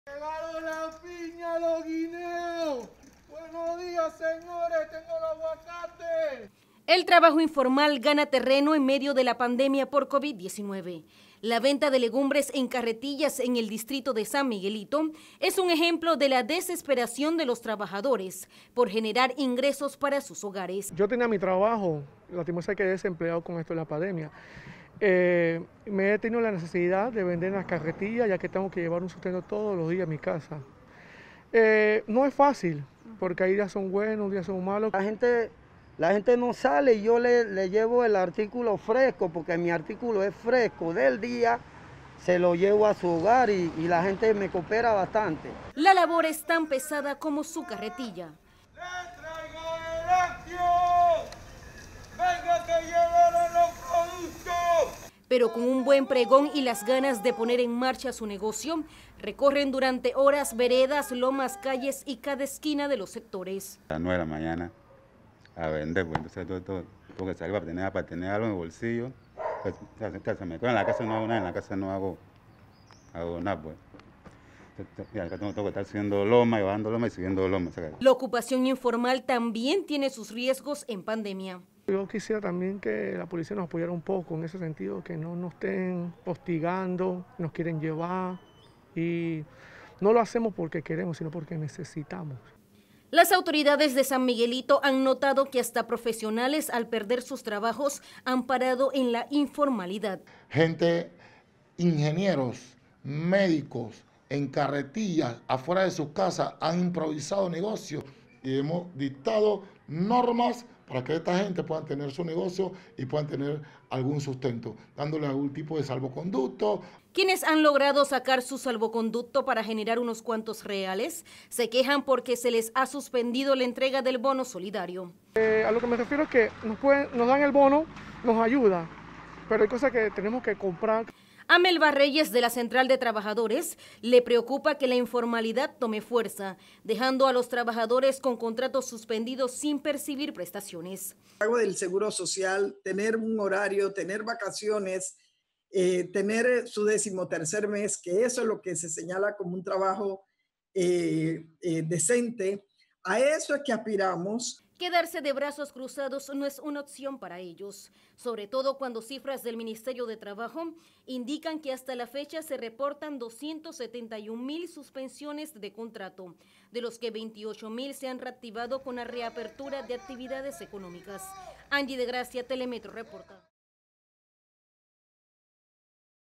Aumenta el trabajo informal en Panamá en medio de pandemia
Decenas de panameños que se han quedado sin trabajo durante la pandemia del COVID-19, se las ingenian para llevar el sustento a su casa, aumentado el trabajo informal.